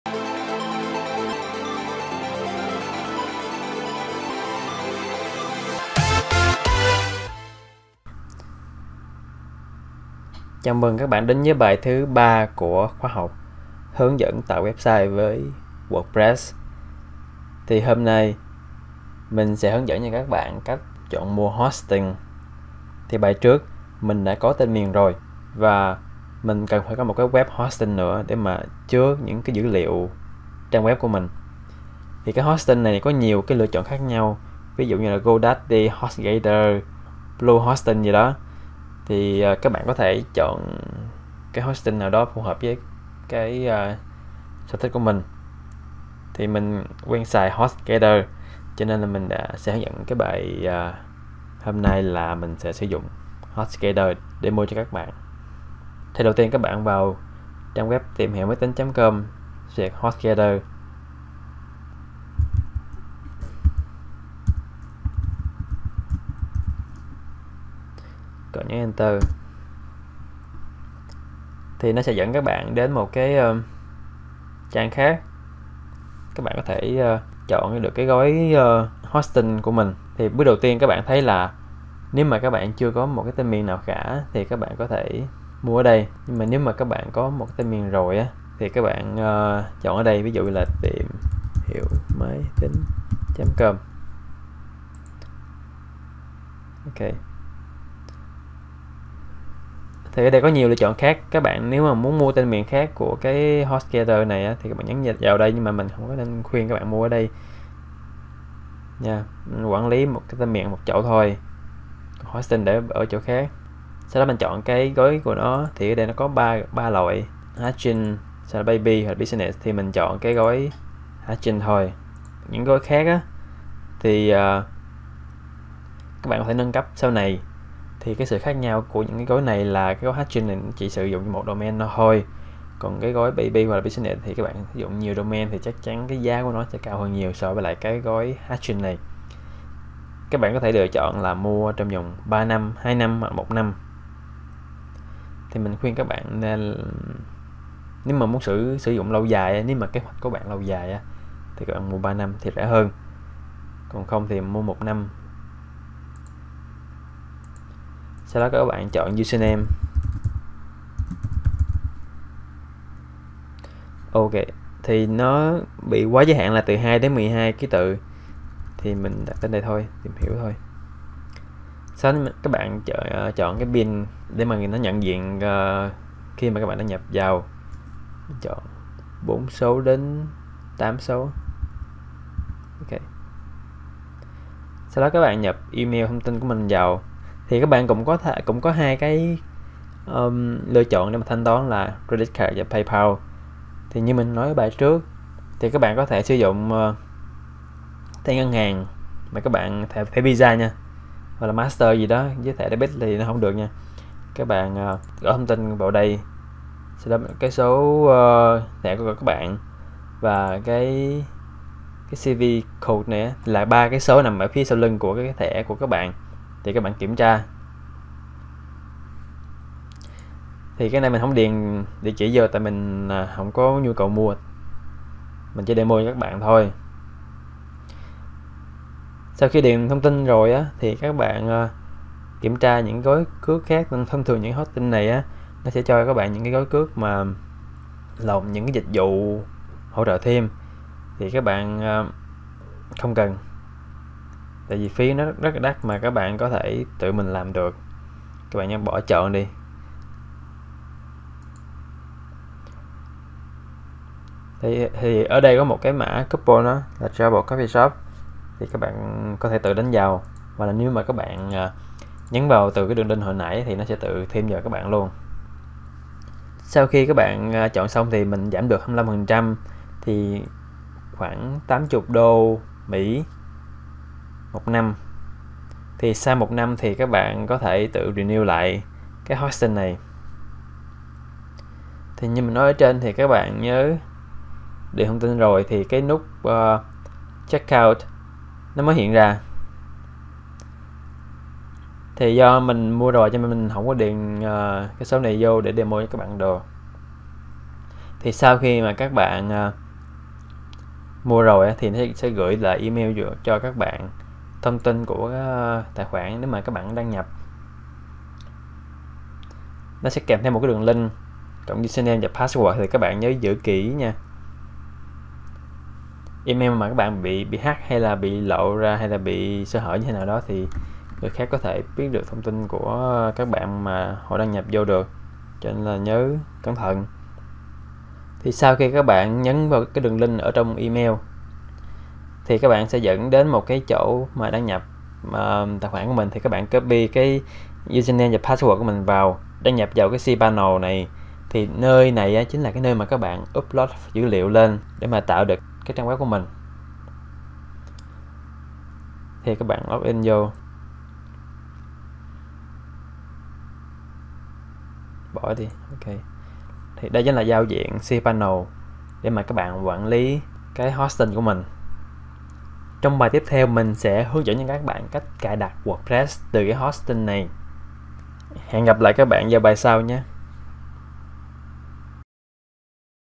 Hướng Dẫn Chọn Mua Hosting - Bài 3
Bài thứ 3 trong loạt bài hướng dẫn thiết kế web bằng WordPress mình sẽ hướng dẫn cho các bạn cách chọn mua và đăng ký hosting cho webiste: